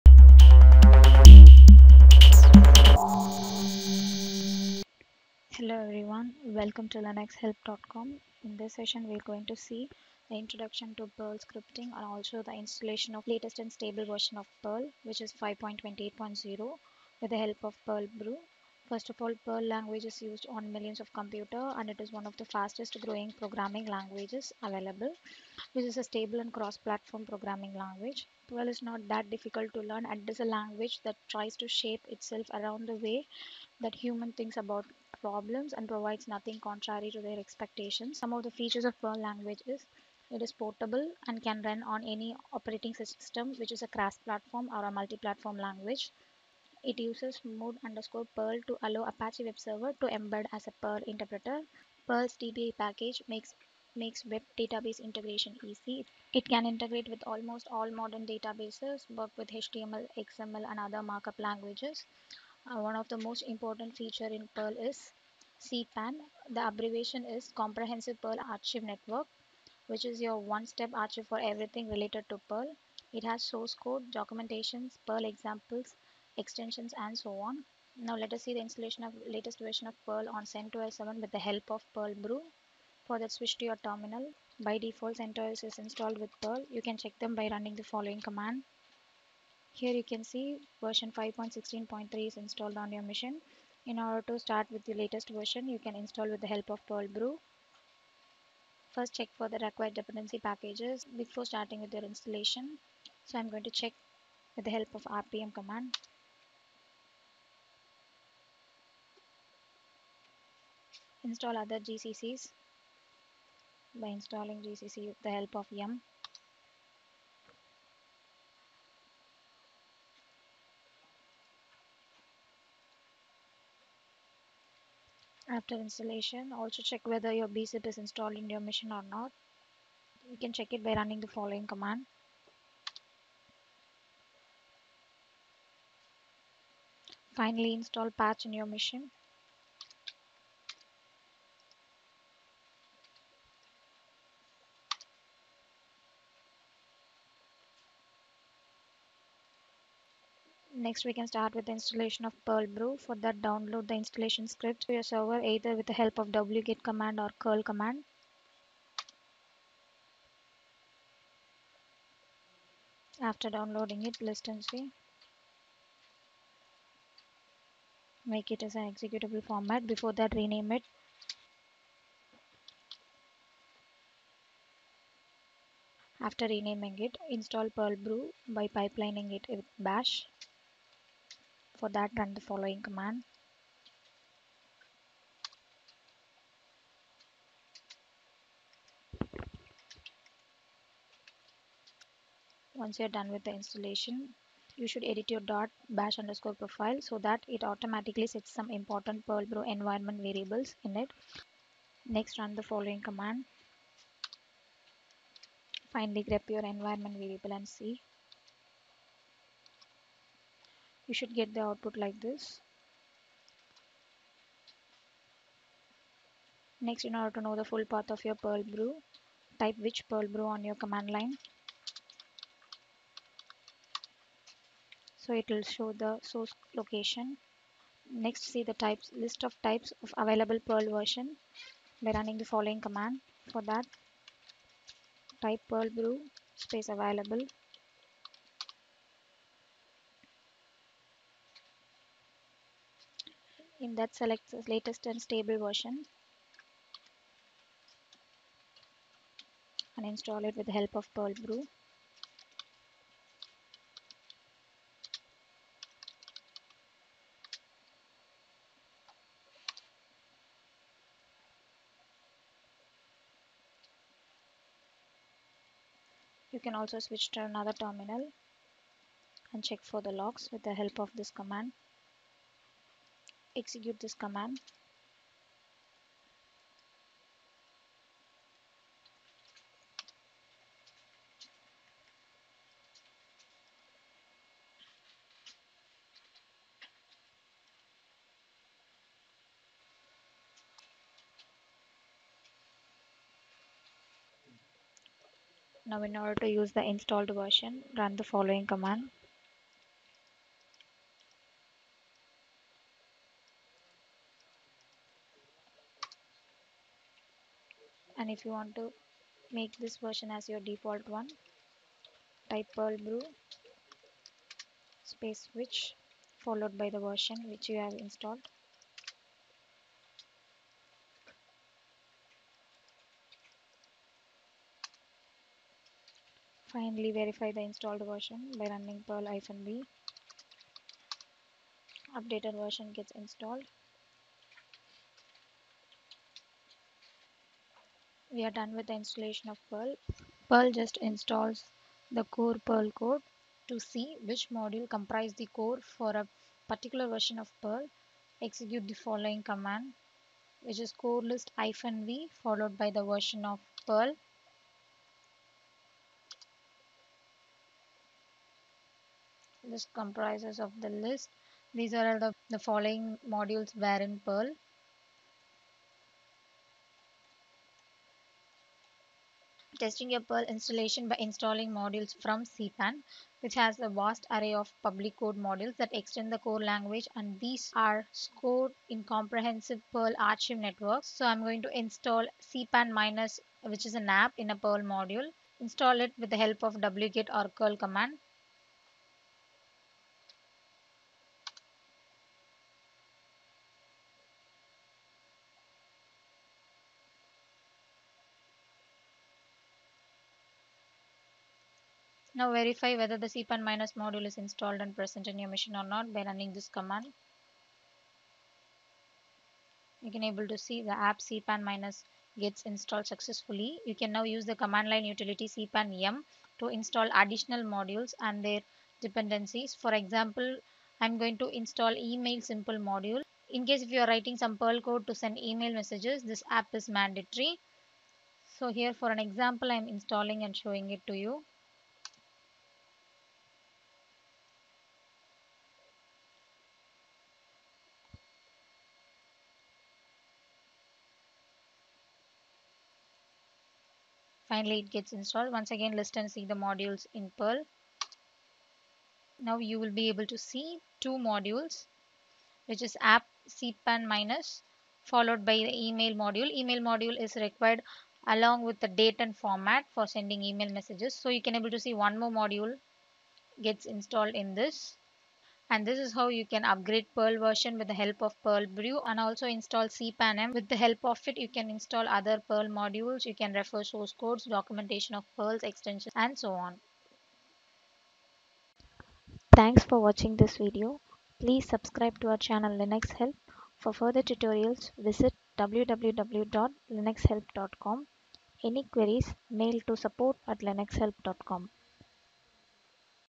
How to install Perl with Perlbrew on CentOS 7
This video covers the method to install Perl with perlbrew on centos 7 and an introduction to Perl Scripting. Perl is used on millions of computers, and it's one of the fastest-growing programming languages available.